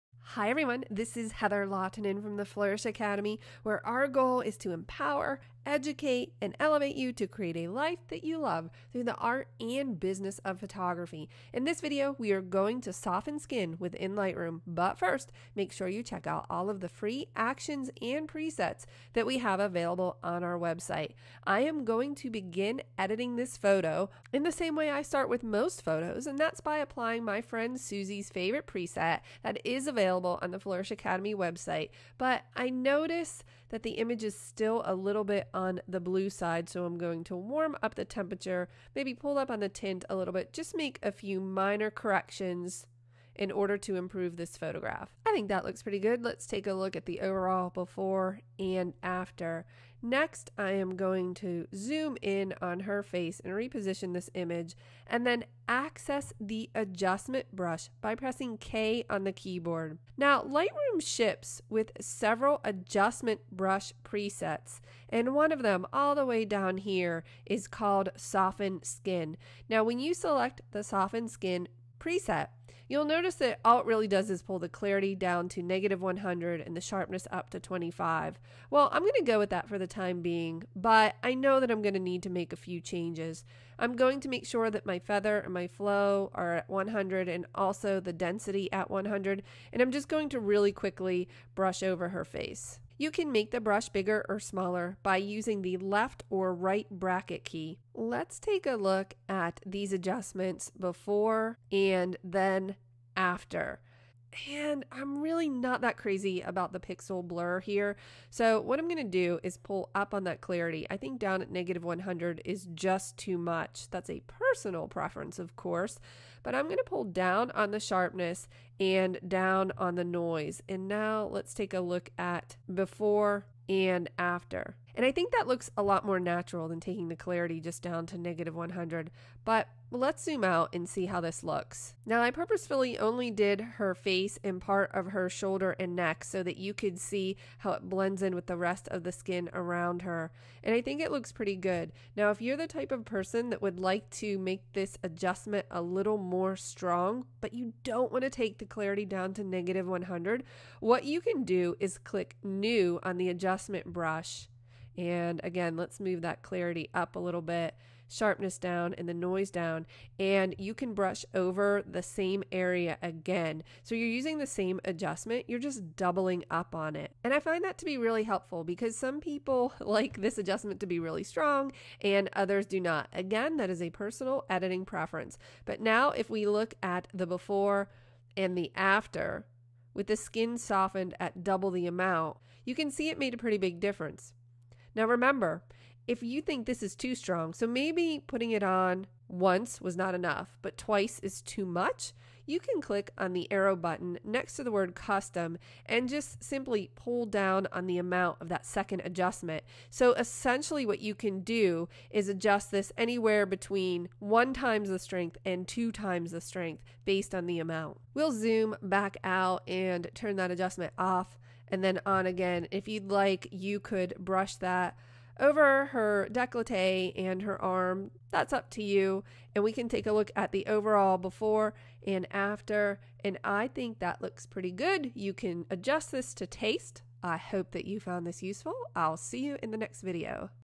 How to Soften Skin in Lightroom
In this video, we take a look at a quick and easy way to soften skin in Lightroom.